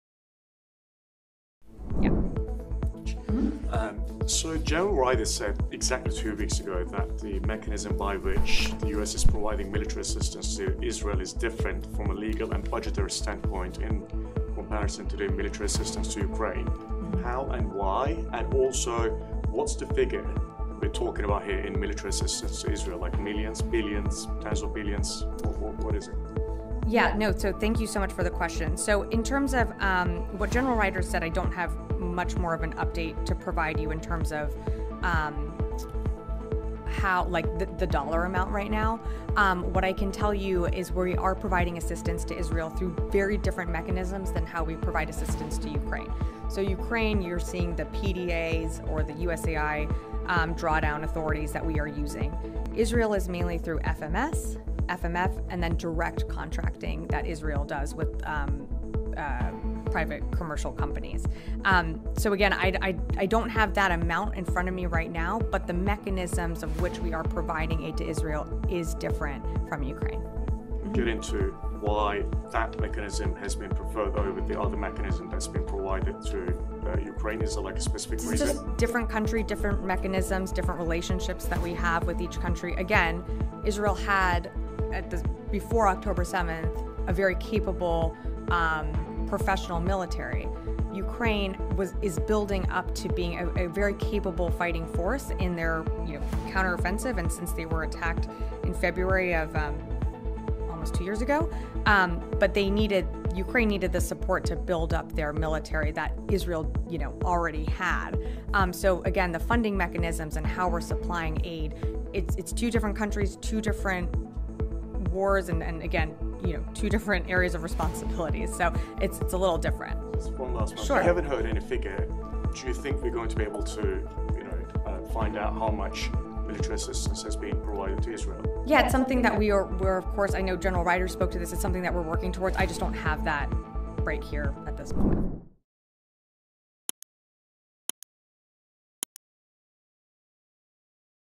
Pentagon Spokesperson Explains US Military Aid To Israel And Ukraine | Dawn News English
During a recent press briefing, Pentagon Spokesperson Sabrina Singh elucidated the United States' military aid strategy to both Israel and Ukraine.

In response to a reporter's inquiry, Singh provided detailed insights into the rationale behind the assistance rendered to these nations.